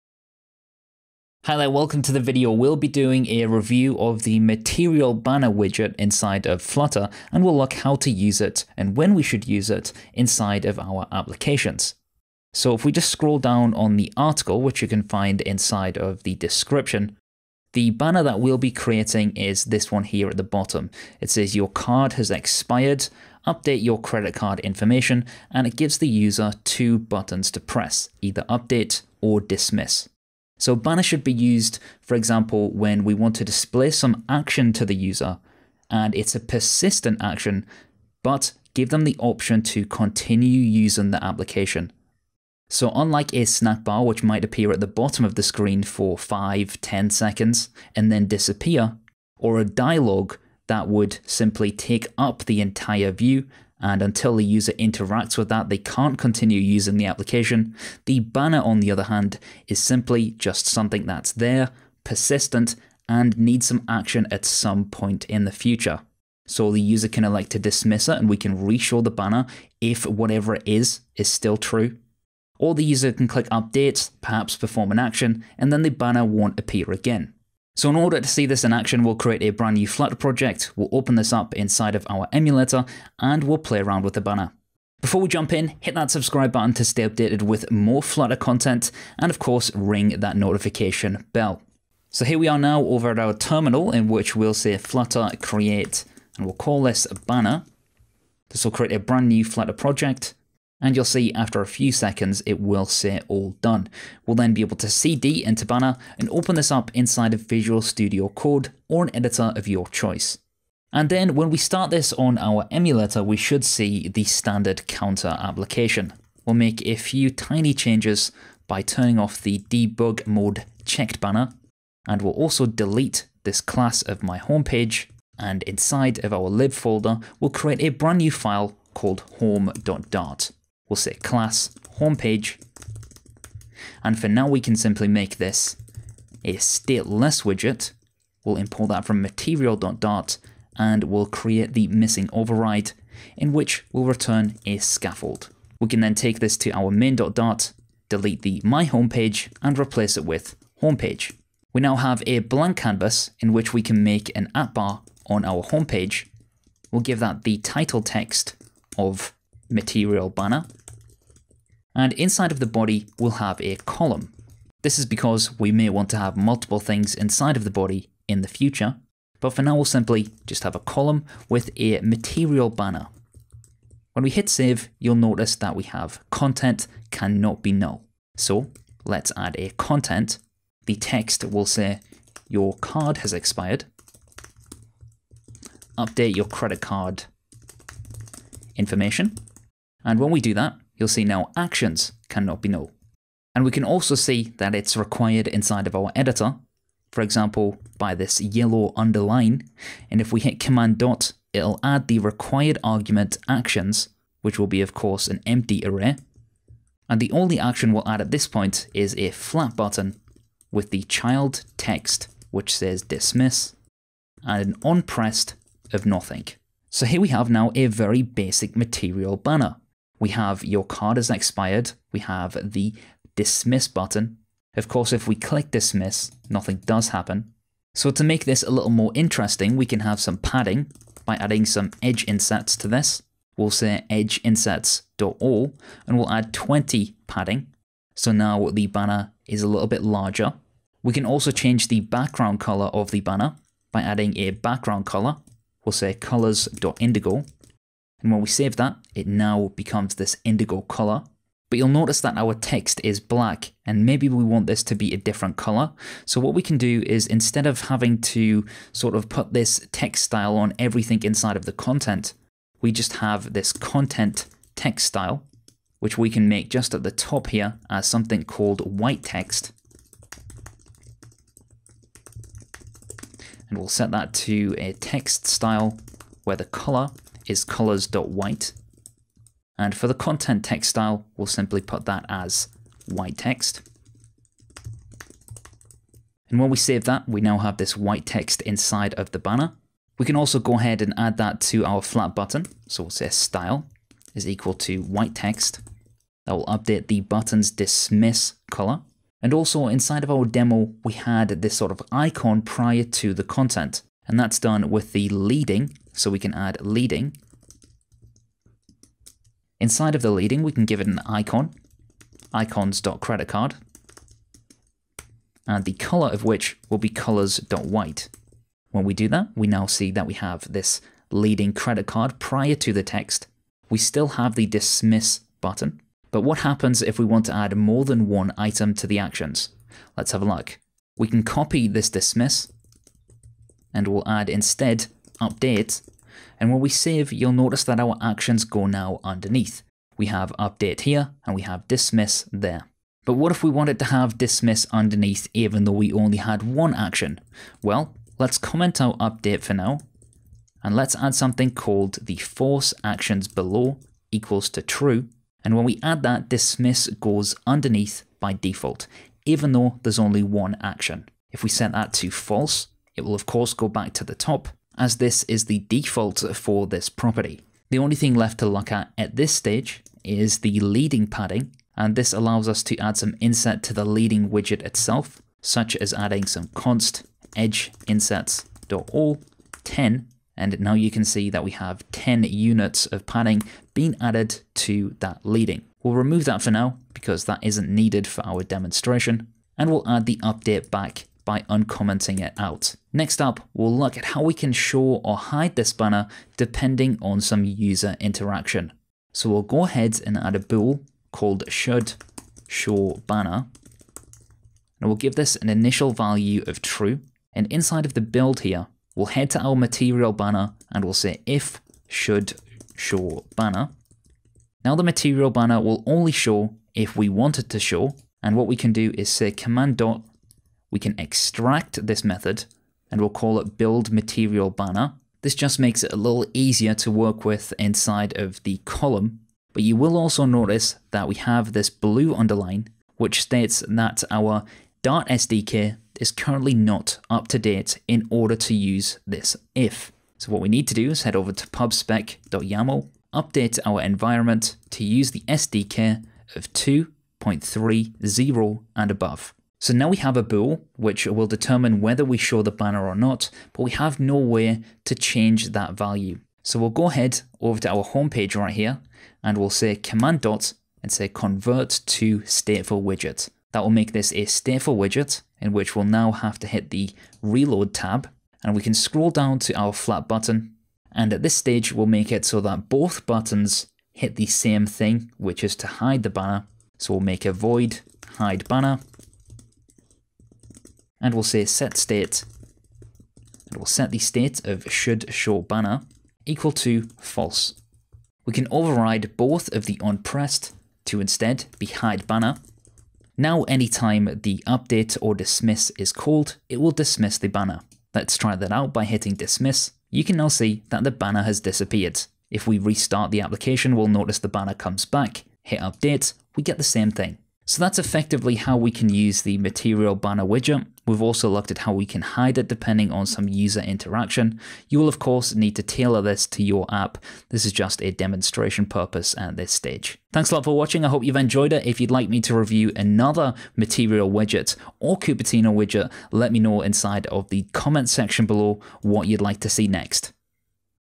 Flutter Widget: MaterialBanner
As part of our Widget review and usage series, we'll be looking at the MaterialBanner Widget within Flutter.

This Widget should be used whenever there's a persistent action that a user should take. As this isn't a Modal or SnackBar the application can still be interacted with, however, the only way to hide this is when the user takes an action.

If you'd like to support the free content, you can do so